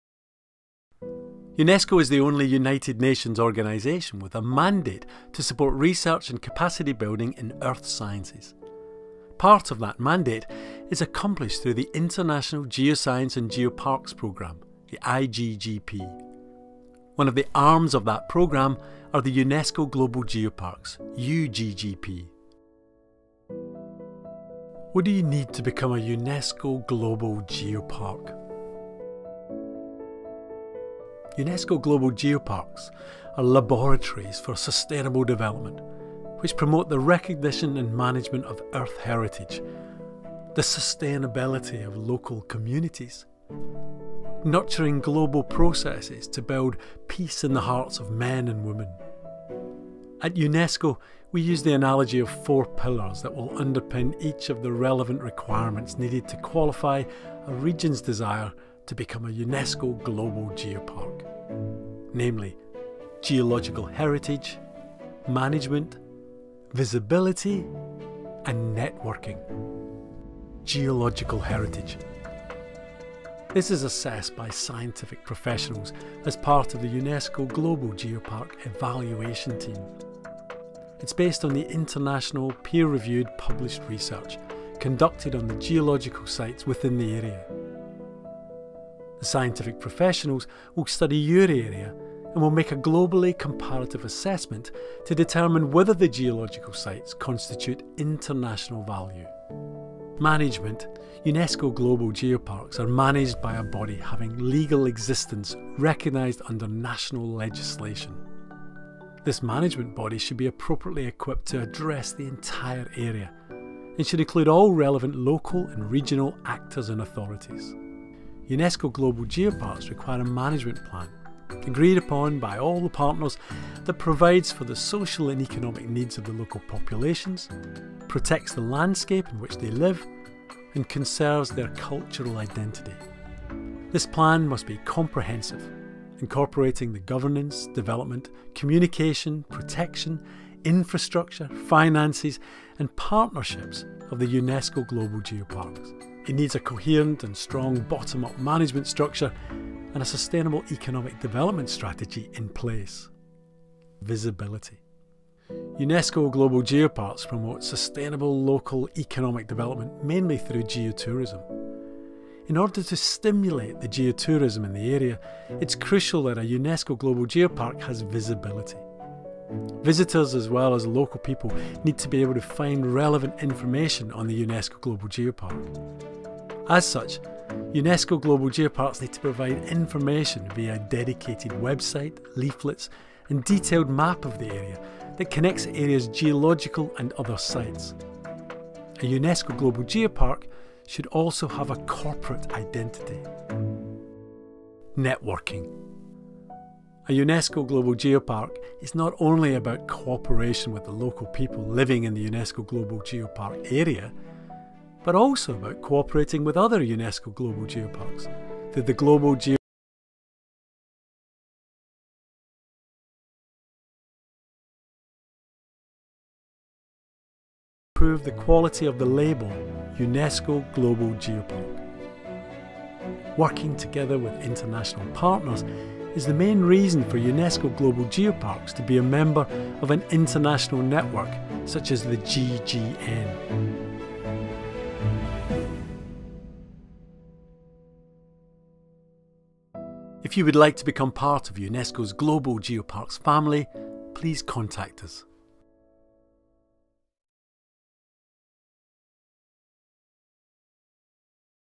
What do you need to become a UNESCO Global Geopark?
Building a Geopark can be an administrative challenge. Indeed, aspiring UNESCO Global Geopark must have geological heritage of international value and be managed by a body having legal existence recognized under national legislation that has a comprehensive management plan, covering governance, development, communication, protection, infrastructure, finance, and partnership issues.